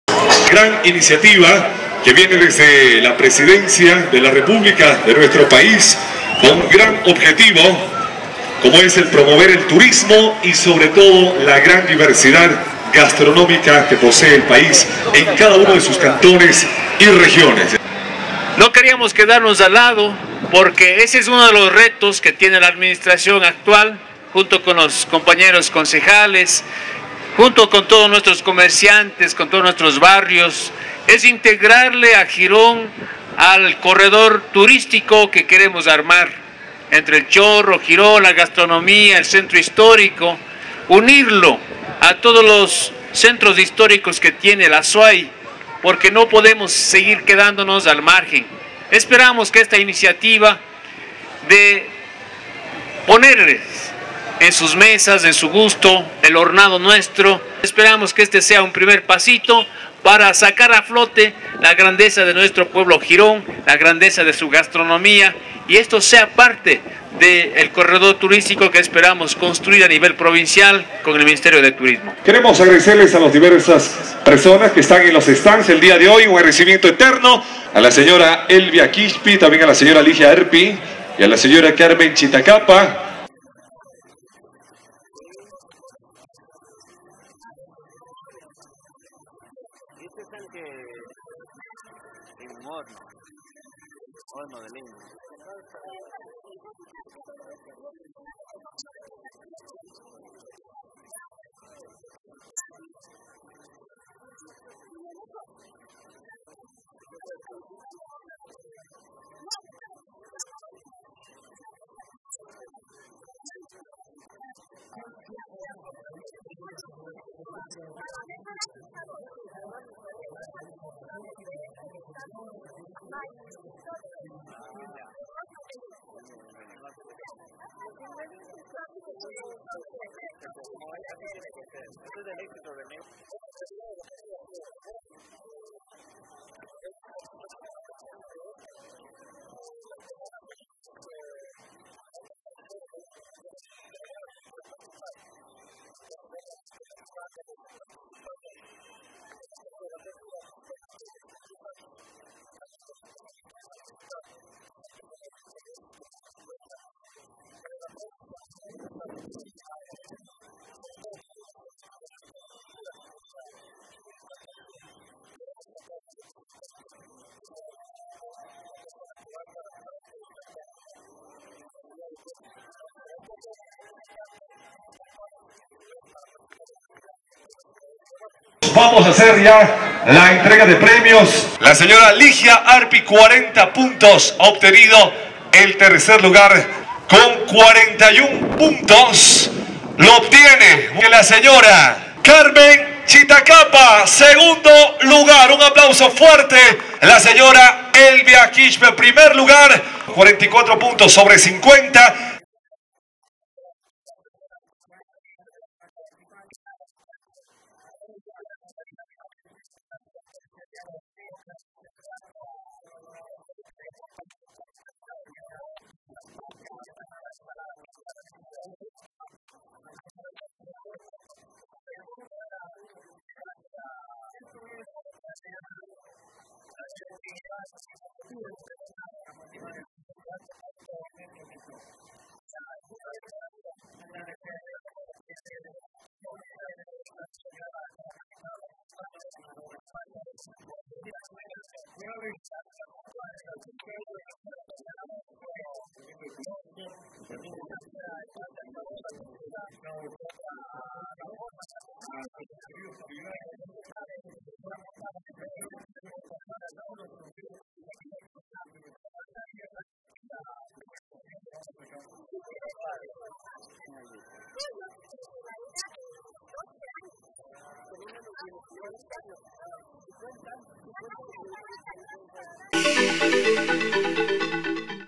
Campeonato del hornado en Girón
El domingo 1 de junio, en las canchas junto al parque infantil, se desarrolló el evento, mediante el cual se nombró a la representante de Girón que participará en la ciudad de Cuenca, en pos de avanzar al "Campeonato Mundial del Hornado" que promueve el Ministerio de Turismo. Tres participantes aceptaron la invitación, fue buena la presencia de público, como también buena fue la venta del chancho hornado. Web